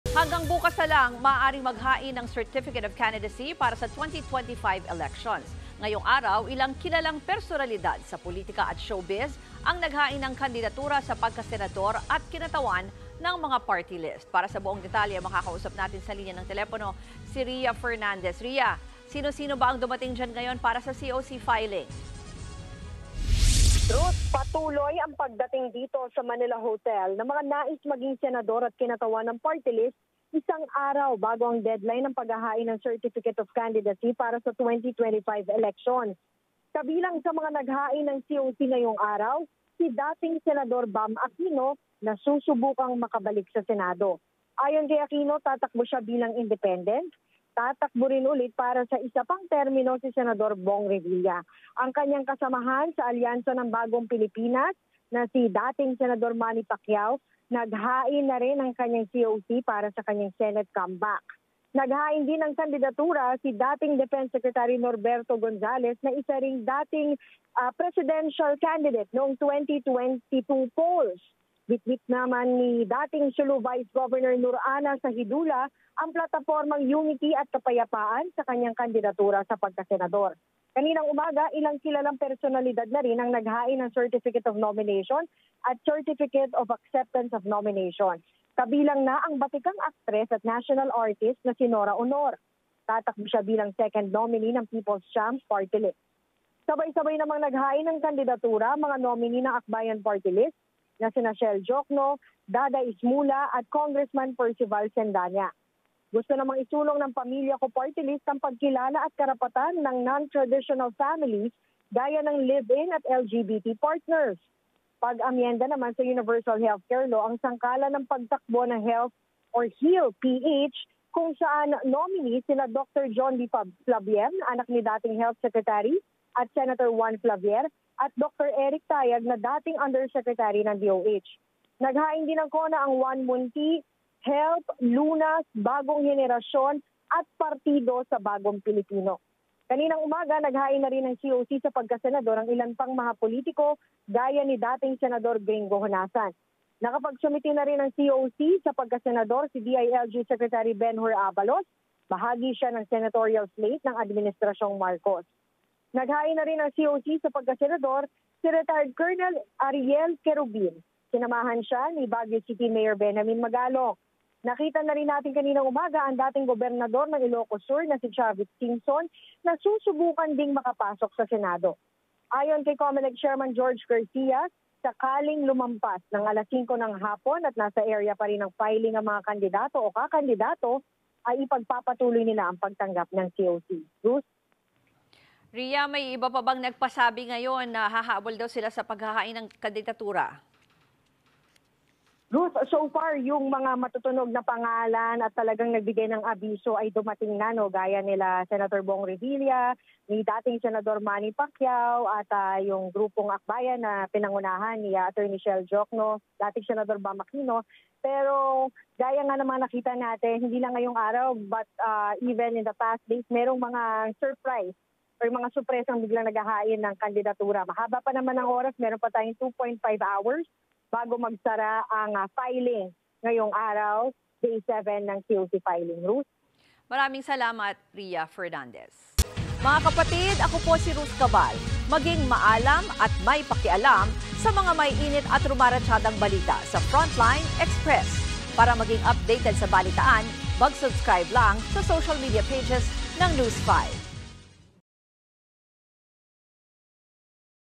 Ilang kilalang personalidad, nakapagsumite na ng kandidatura para sa 2025 elections
FrontlineExpress | Ilang kilalang personalidad sa pulitika at showbiz ang naghain ng kandidatura sa pagkasenador at kinatawan ng mga party-list ngayong Lunes, Oct. 7.

Samantala, hanggang bukas, Oct. 8, na lang maaaring maghain ng certificate of candidacy para sa 2025 elections. BilangPilipino2025 News5 | via Ria Fernandez-Obina